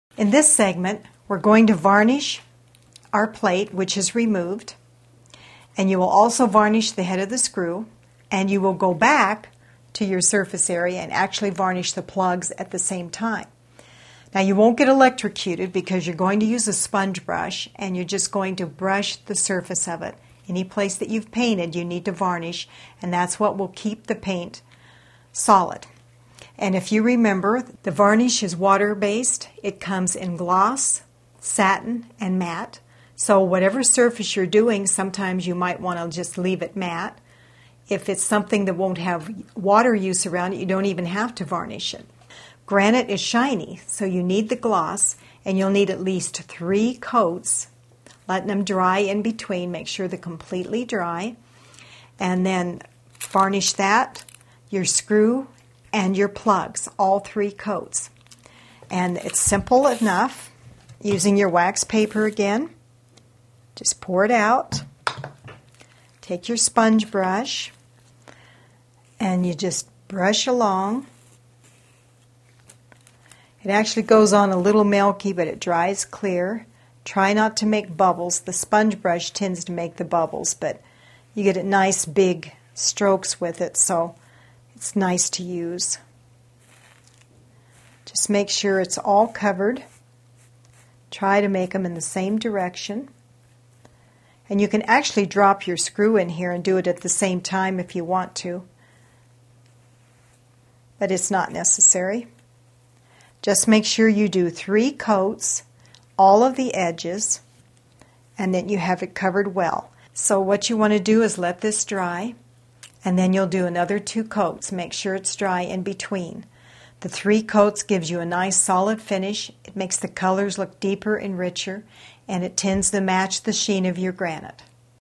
Painting a Wall Outlet for Granite: Applying Varnish
Painting a Wall Outlet for Granite: Applying Varnish. Part of the series: Painting Wall Outlets for Granite Surfaces. The screw on the wall outlet should be varnished along with the surface. Learn how to varnish a wall plate from an expert artist in this free painting video.